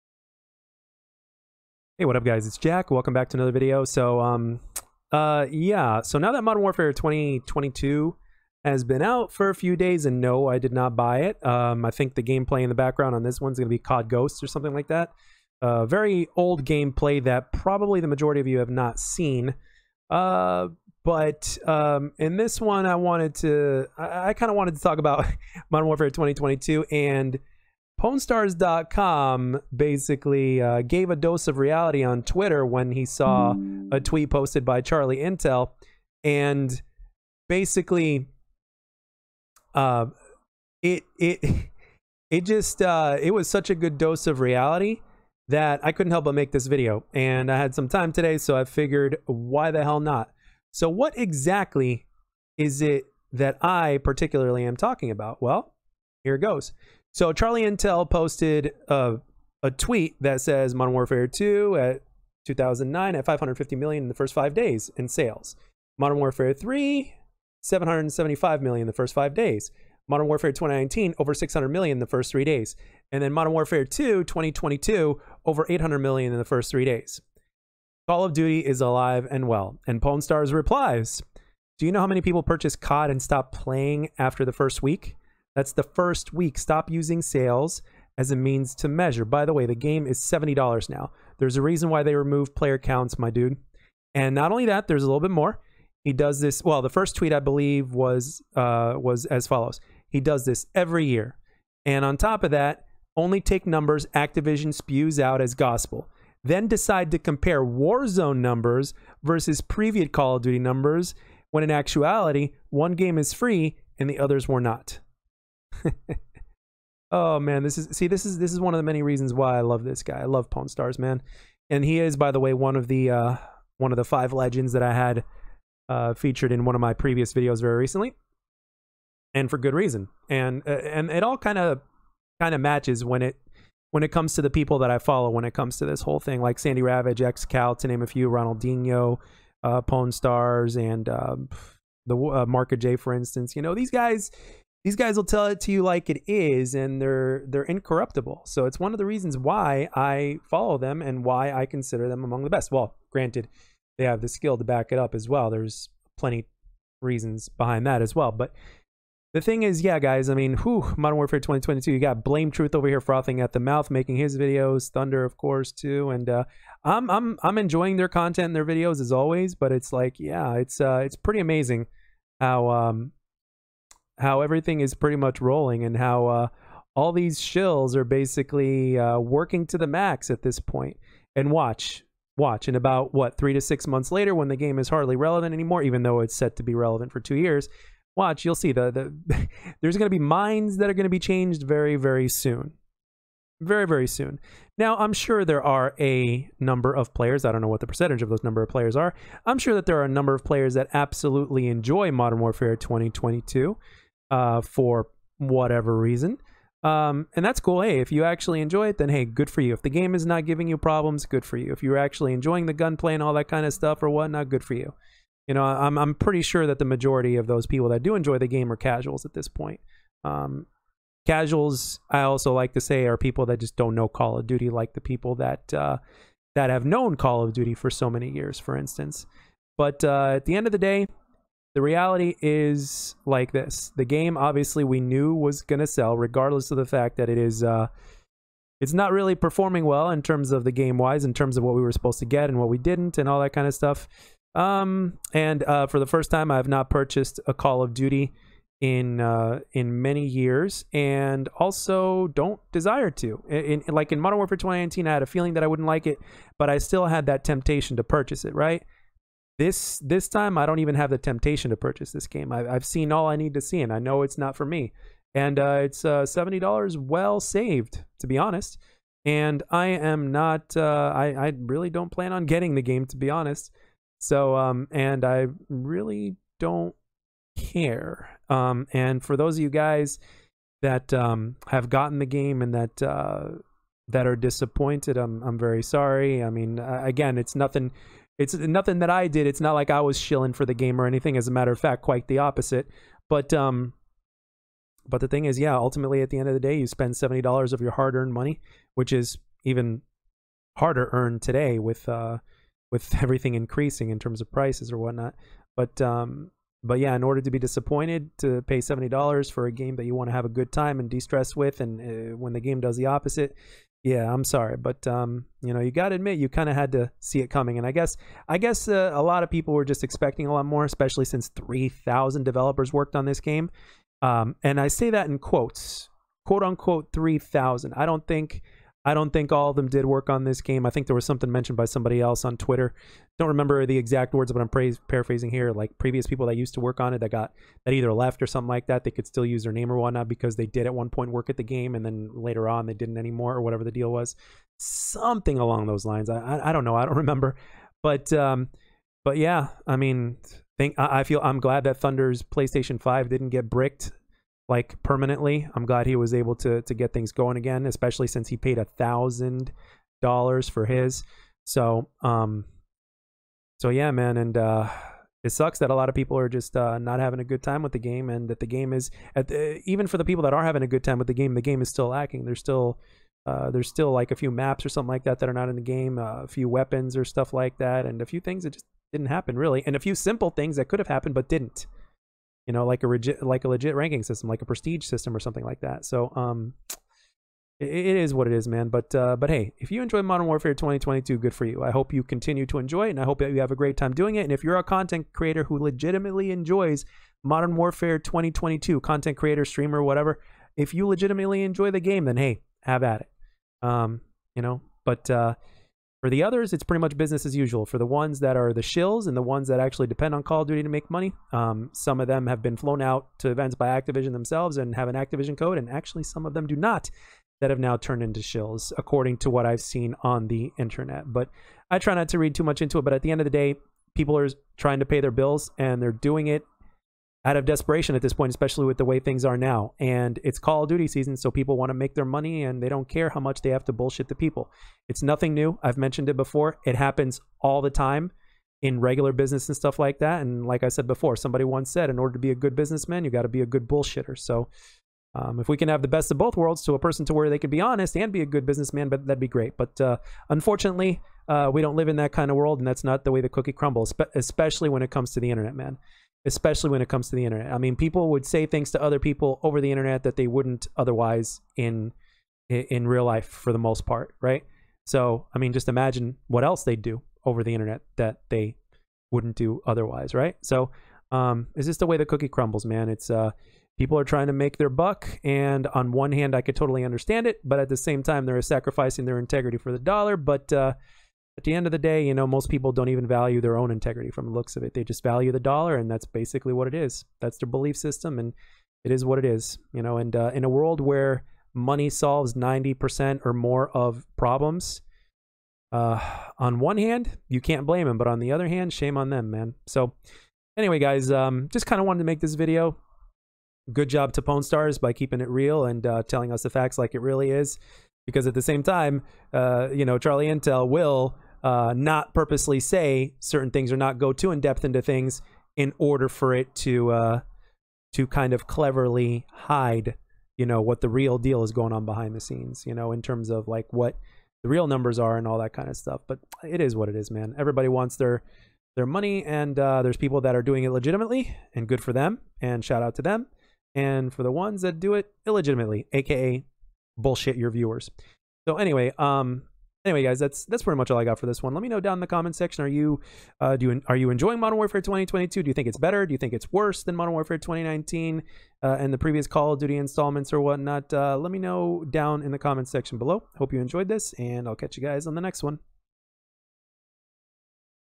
"Call of Duty Is Alive and Well"
According to CharlieIntel, Call of Duty is alive and well. But is it really? Enjoy.